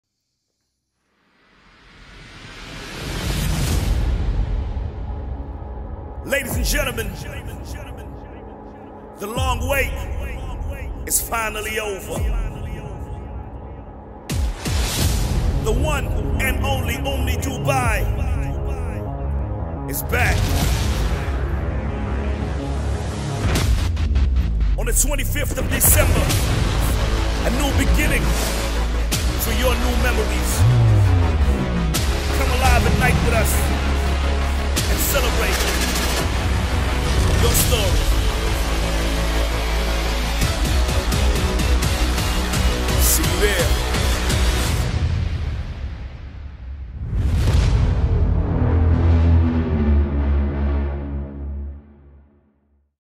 OMNI DUBAI 20201 - REOPENING
OMNI DUBAI is coming back & in a new concept, with live Entertainment shows that will blow your mind, stay tuned with us for the opening on 25th of December.. it is jus the beginning!